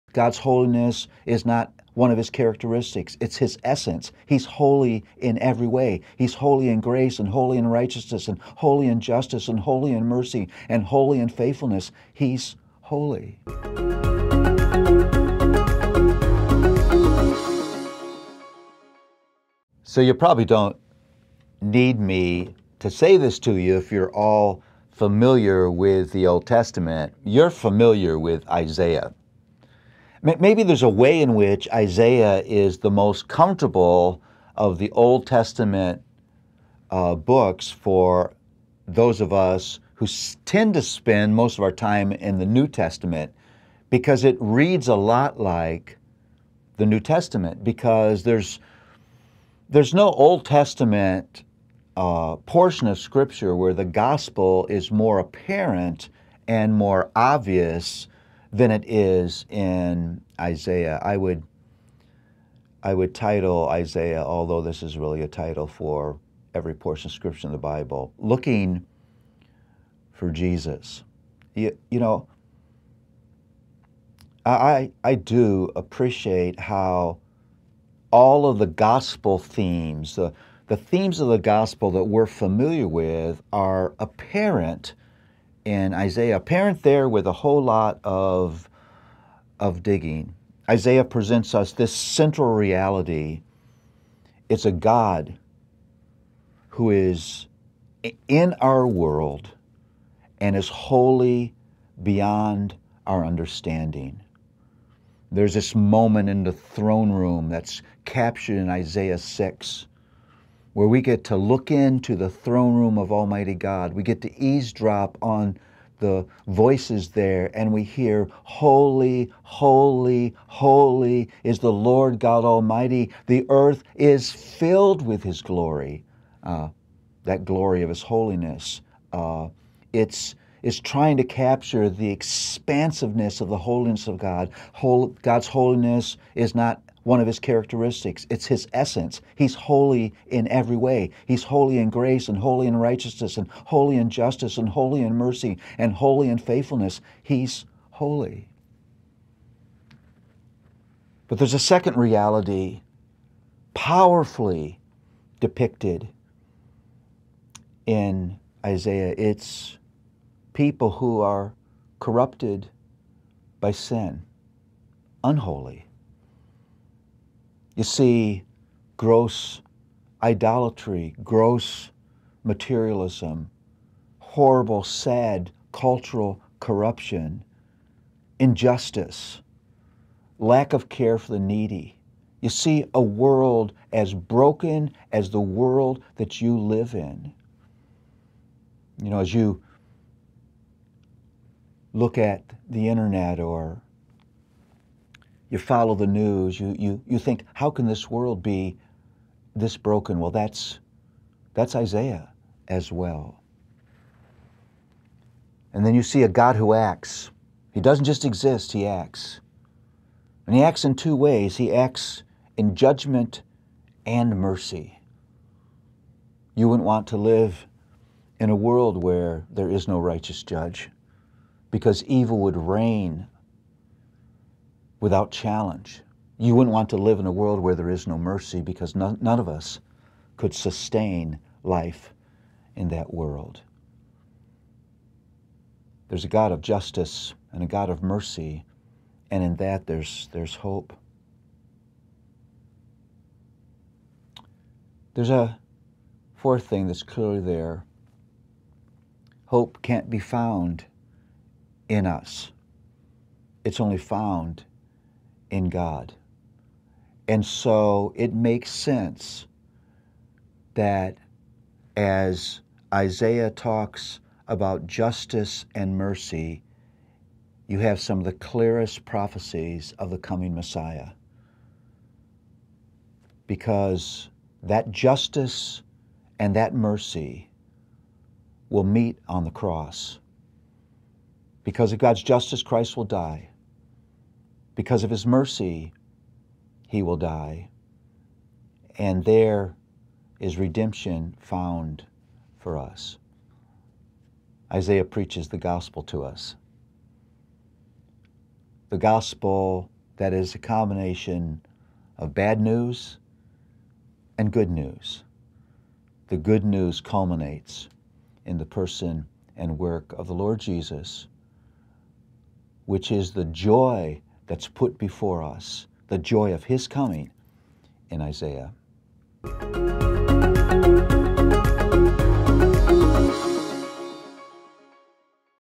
Isaiah Summary - Paul Tripp's Bible Study (Episode 024)
Isaiah: Looking For Jesus. Pastor and best-selling author Dr. Paul David Tripp continues his new weekly Bible Study by summarizing the book of Isaiah.